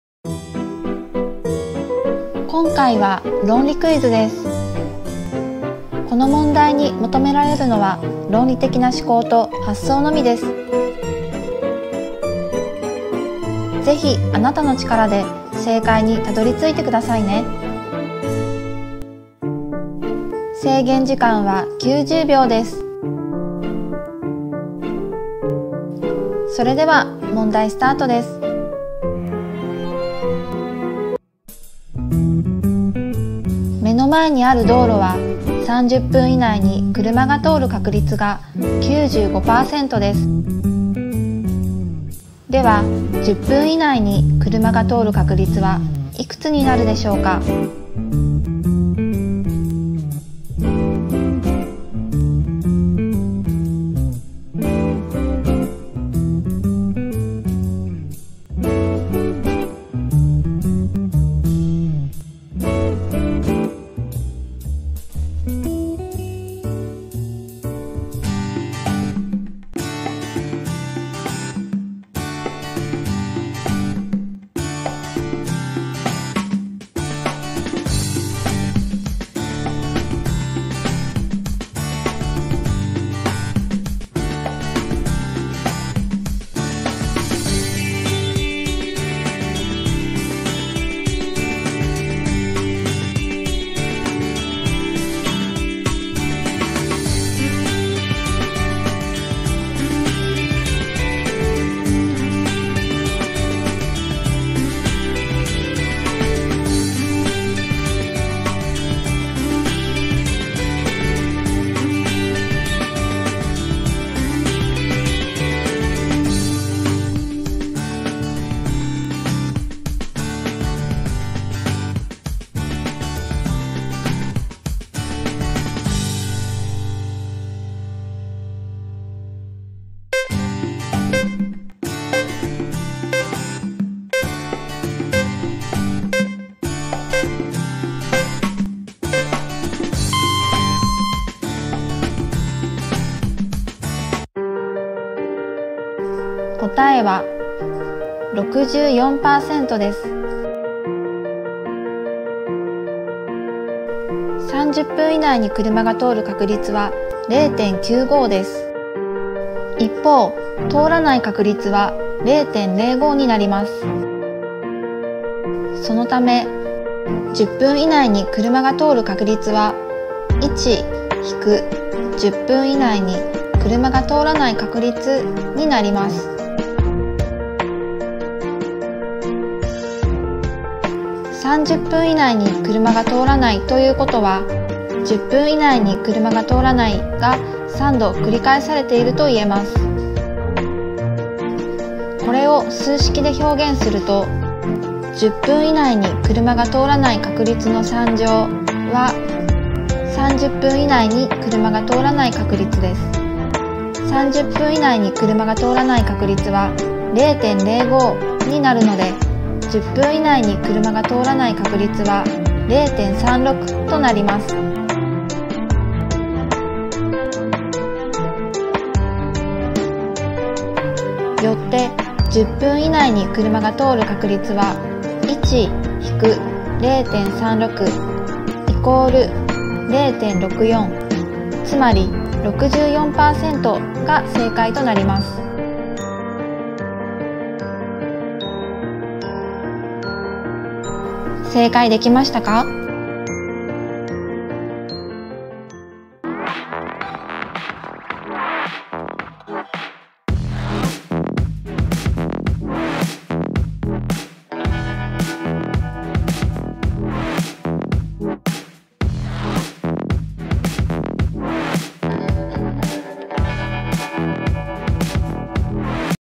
🔵計算が苦手な人は解けないクイズ🔴30分内に車が通る確率は95％！さて…10分以内は何％になる？
今回は、問題文を読んで考えれば論理的に答えがでる論理クイズです！
「目の前にある道路は、30分以内に
車が通る確率が、95％です。では、10分以内に車が通る確率はいくつでしょうか？」

数学的な思考が必要な問題ですよ。
制限時間は90秒です！落ち着いて考えてみてくださいね。

論理クイズから恋愛マスターになるクイズなど年齢を問わず楽しめるズッキュ～ンするクイズを出題中。